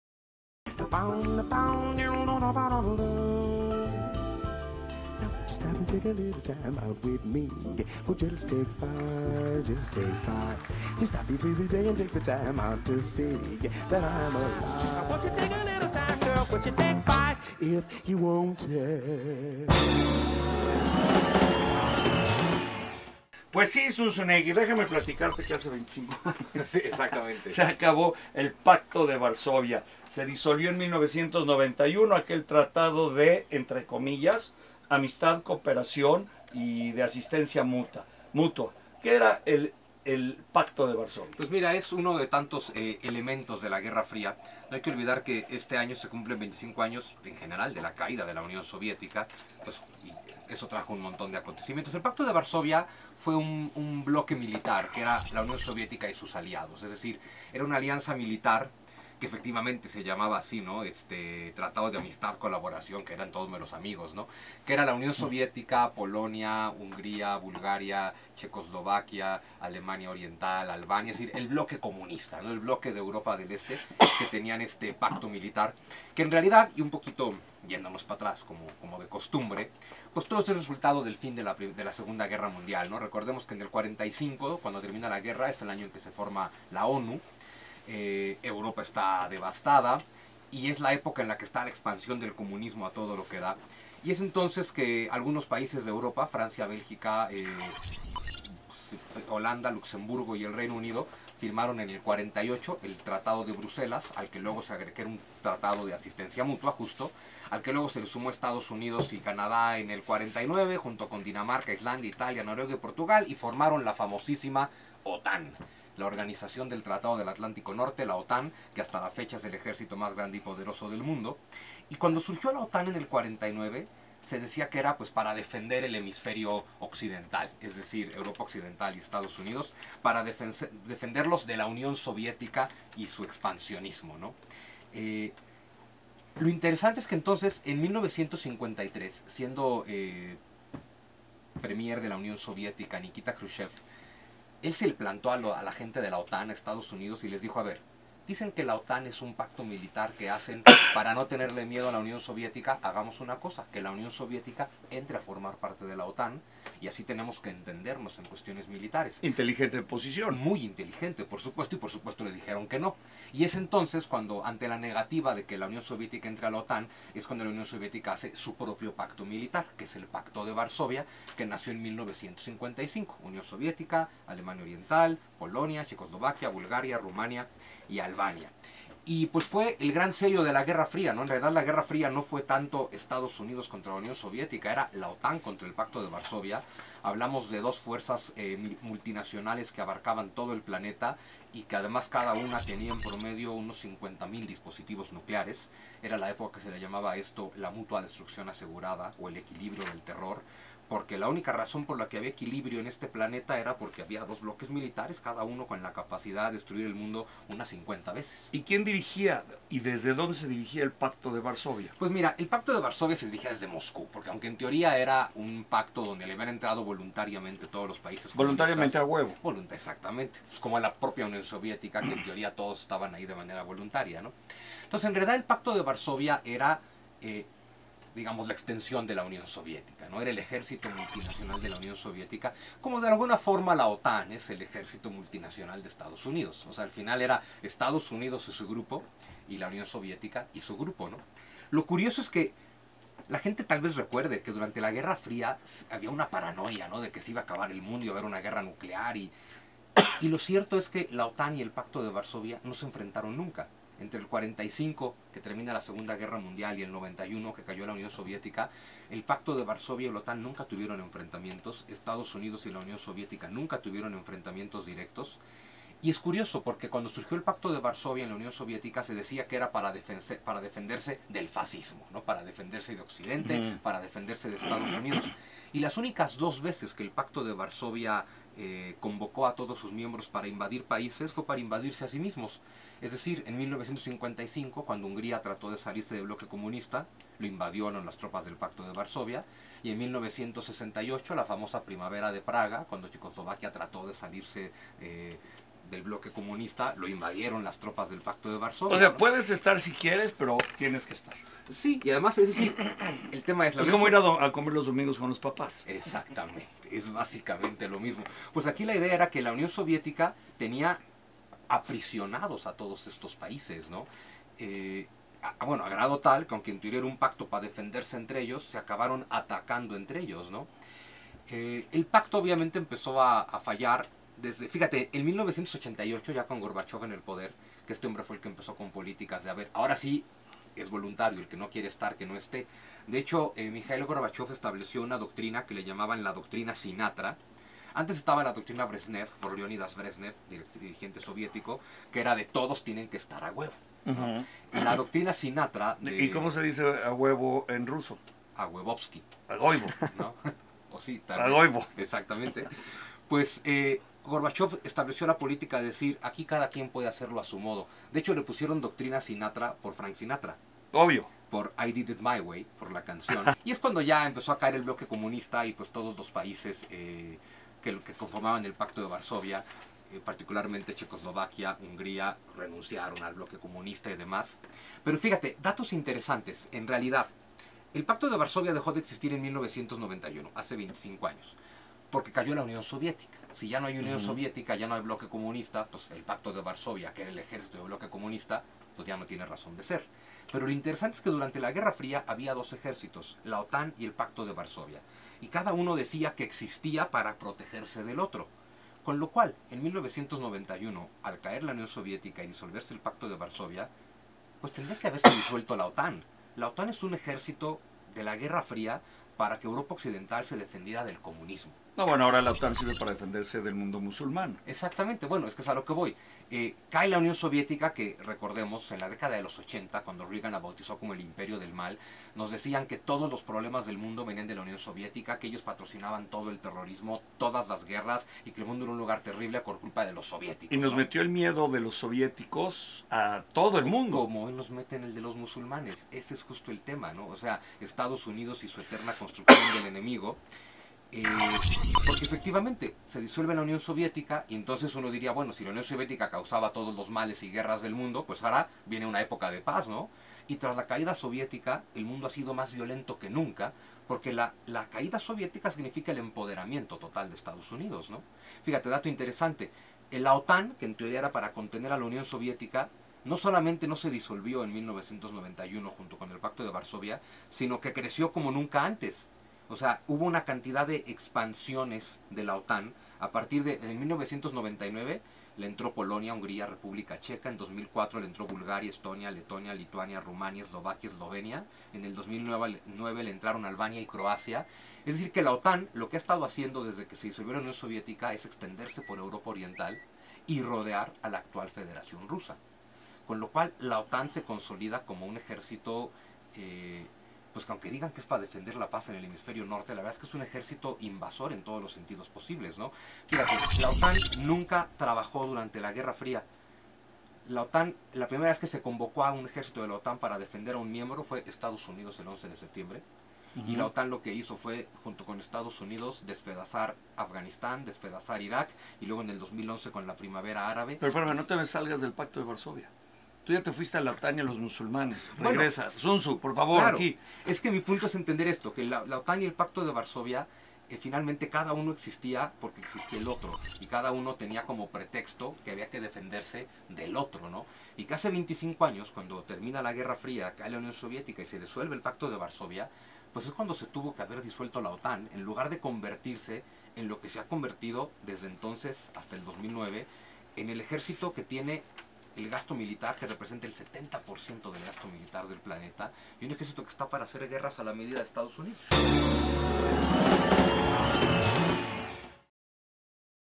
El pacto de Varsovia, retrospectiva a 25 años de su fin en voz de Zunzu
Entendamos el fin del pacto de varsovia y el expansionismo de su rival que tuvo que haber muerto con el: la OTAN